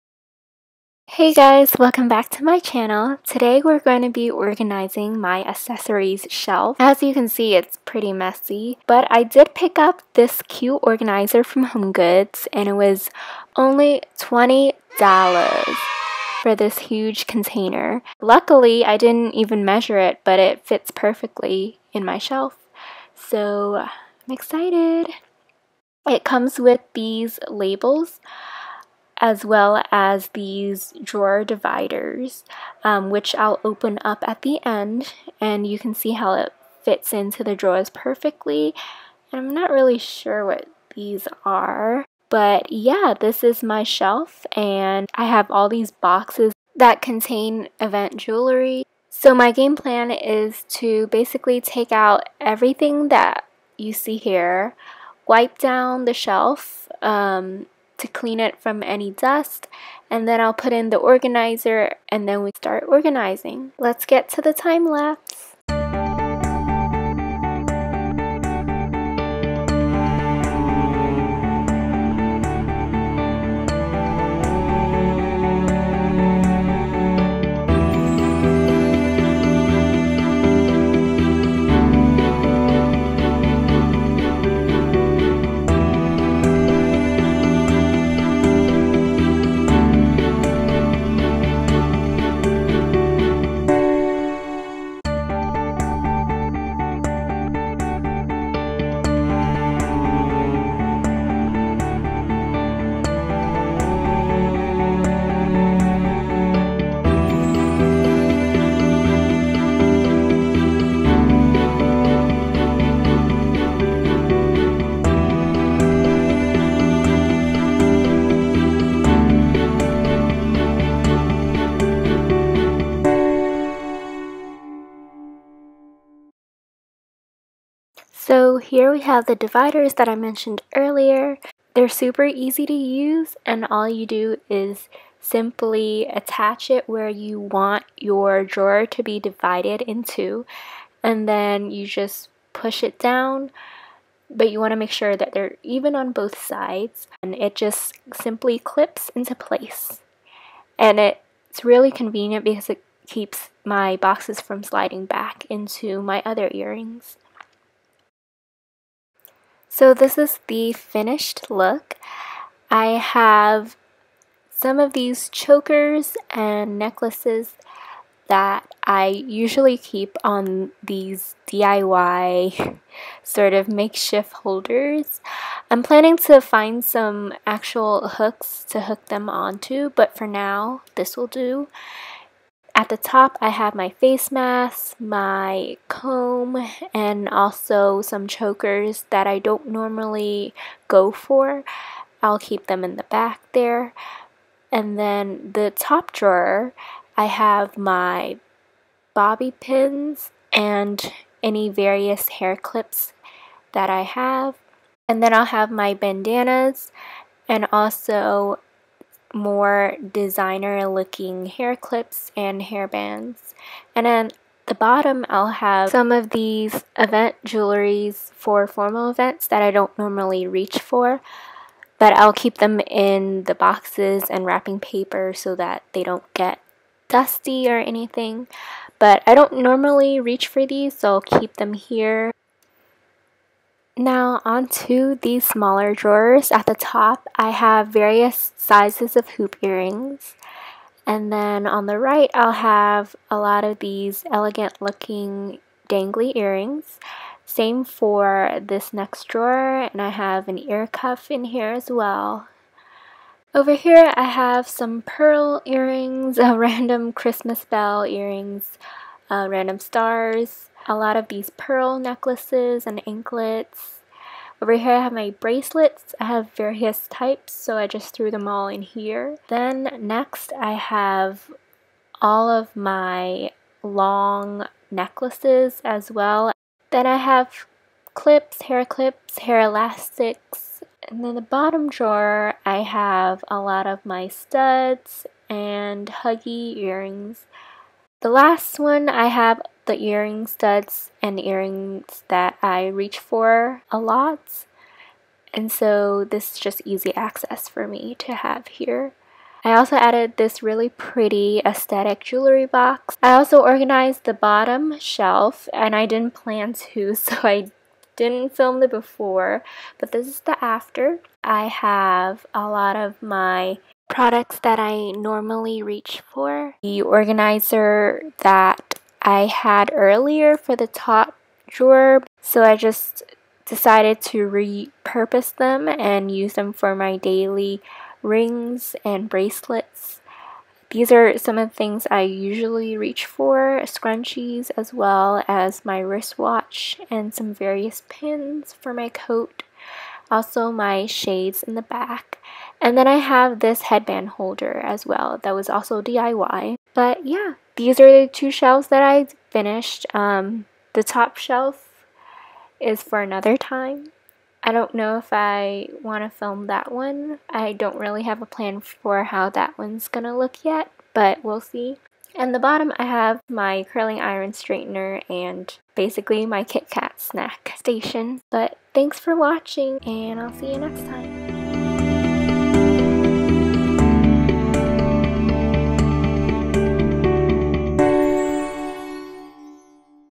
organize my accessories with me || cindyy_rella
Hi guys! Today I finally plan to organize my disaster of an accessories shelf & I bring you guys along for this journey uwu.

Small Parts Organizer - Home Goods